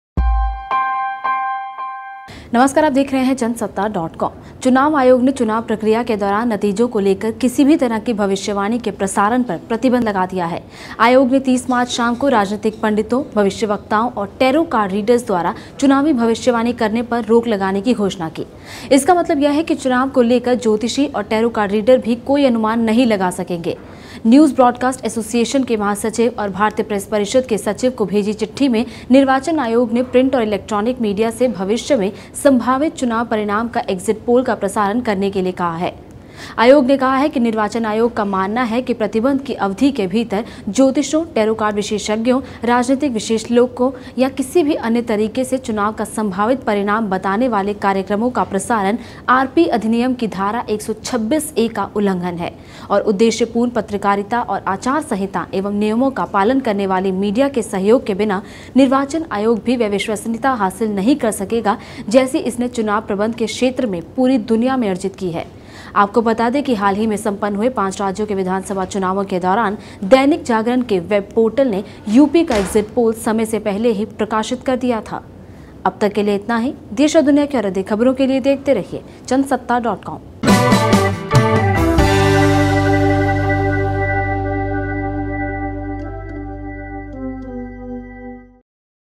Election Commission Tells Media To Not Predict Polls Using Astrologers
चुनाव आयोग ने चुनाव प्रक्रिया के दौरान नतीजों को लेकर किसी भी तरह की तरह की भविष्‍यवाणी के प्रसारण पर प्रतिबंध लगाया दिया है। आयोग ने 30 मार्च शाम को राजनैतिक पंडितों, भविष्‍यवक्‍ताओं और टैरो कार्ड रीडर्स द्वारा चुनावी भविष्‍यवाणी करने पर रोक लगाने की घोषणा की। इसका मतलब यह है कि चुनाव को लेकर ज्‍योतिषी और टैरो कार्ड रीडर भी कोई अनुमान नहीं लगा सकेंगे। न्यूज ब्रॉडकॉस्टर्स एसोसिएशन के महासचिव और भारतीय प्रेस परिषद के सचिव को भेजी चिट्ठी में निर्वाचन आयोग ने प्रिंट और इलेक्ट्रॉनिक मीडिया से भविष्य में संभावित चुनाव परिणाम या एक्जिट पोल का प्रसारण न करने के लिए कहा है। आयोग ने कहा है कि निर्वाचन आयोग का मानना है कि प्रतिबंध की अवधि के भीतर ज्योतिषियों, टैरो कार्ड विशेषज्ञों, राजनीतिक विश्लेषकों या किसी भी अन्य तरीके से चुनाव का संभावित परिणाम बताने वाले कार्यक्रमों का प्रसारण आर. पी. अधिनियम की धारा 126ए का उल्लंघन है।” और ‘उद्देश्यपूर्ण पत्रकारिता और आचार संहिता एवं नियमों का पालन’ करने वाली मीडिया के सहयोग के बिना निर्वाचन आयोग भी ‘वह विश्वसनीयता हासिल नहीं कर सकेगा, जैसी इसने चुनाव प्रबंधन के क्षेत्र में पूरी दुनिया में अर्जित की है’। आपको बता दें कि हाल ही में संपन्‍न हुए पांच राज्‍यों के विधानसभा चुनाव के दौरान दैनिक जागरण के वेब-पोर्टल ने यूपी का एग्जिट पोल समय से पहले ही प्रकाशित कर दिया था। जागरण के ऑनलाइन संपादक शंशाक शेखर त्रिपाठी को इस मामले में गिरफ्तार किया गया था और बाद में उन्हें स्थानीय कोर्ट में जमानत मिलने पर छोड़ दिया गया। Jansatta is one of the leading hindi news channel, which provides all the latest and trending news of india and around the world.